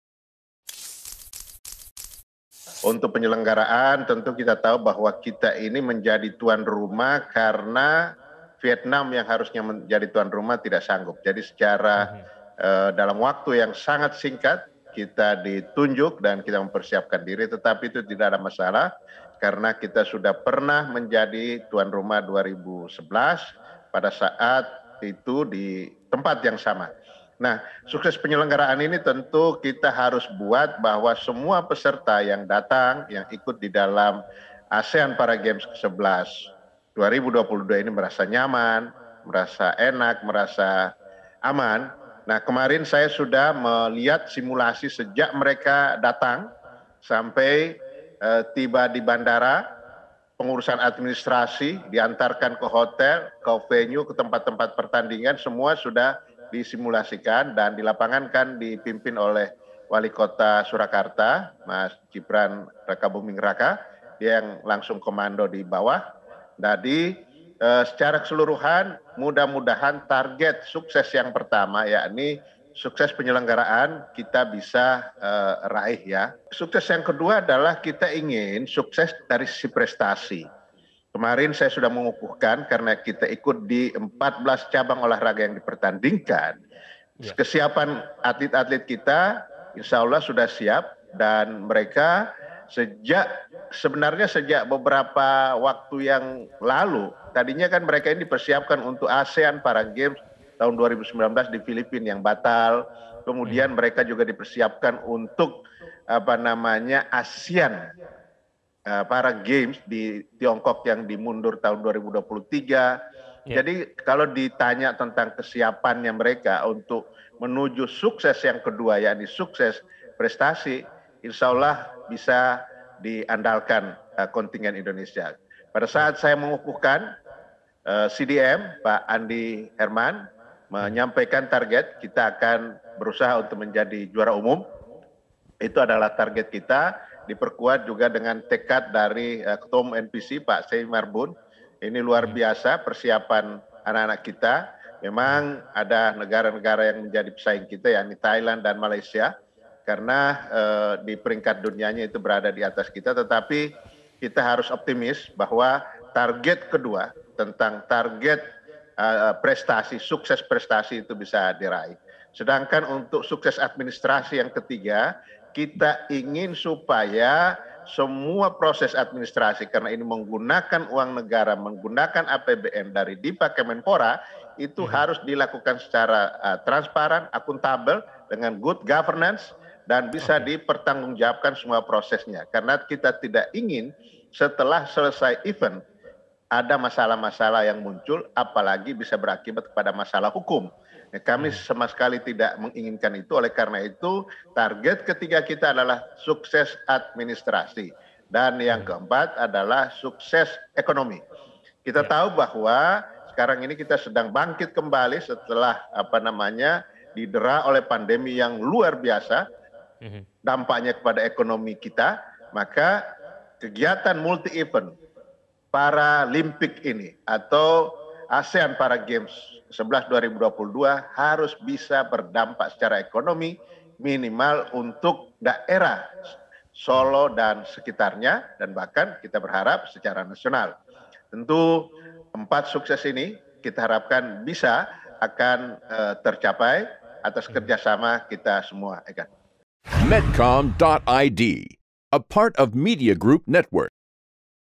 Jelang ASEAN Para Games 2022, Menpora Targetkan 4 Sukses
Jelang ASEAN Para Games 2022, Menpora Zainudin Amali mencanangkan 4 sukses, yakni sukses penyelenggaraan, sukses prestasi, sukses administrasi dan sukses ekonomi. Hal ini dinilai penting agar event ASEAN Para Games 2022 tidak hanya mengangkat nama Indonesia di mata negara-negara Asia Tenggara, tetapi juga memberikan dampak ekonomi bagi masyarakat sekitar dan tertib administrasi bagi pemerintah.